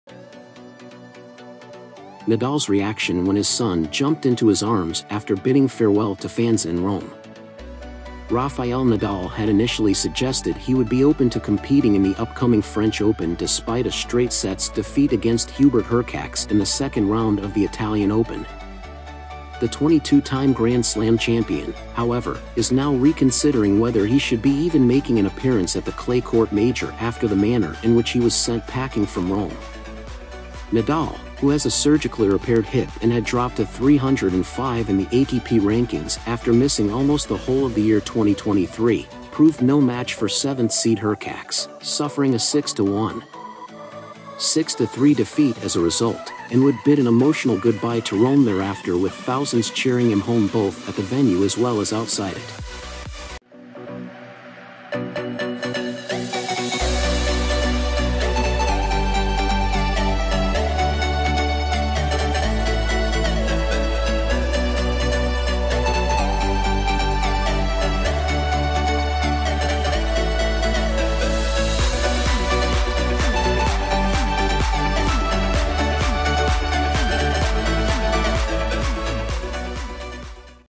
Nadal's Reaction When His Son Jumped into His Arms after Bidding Farewell to Fans in Italian Open 😊😊
Rafael Nadal had initially suggested he would be open to competing in the upcoming French Open despite a straight sets defeat against Hubert Hurkacz in the second round of the Italian Open. The 22-time Grand Slam champion, however, is now reconsidering whether he should be even making an appearance at the clay-court Major after the manner in which he was sent packing from Rome.

Nadal, who has a surgically-repaired hip and had dropped to 305 in the ATP rankings after missing almost the whole of the year 2023, proved no match for seventh-seed Hurkacz, suffering a 6-1, 6-3 defeat as a result, and would bid an emotional goodbye to Rome thereafter with thousands cheering him home both at the venue as well as outside it.

🍂 𝙈𝙚𝙡𝙖𝙮𝙖𝙣𝙞 𝙅𝙖𝙨𝙖 𝙋𝙚𝙢𝙗𝙪𝙖𝙩𝙖𝙣 𝙋𝙋𝙏
(Bahan mengajar, Presentasi, Template Keagamaan, dll)
Harga Mulai 5K. MAU ORDER, klik link: ➭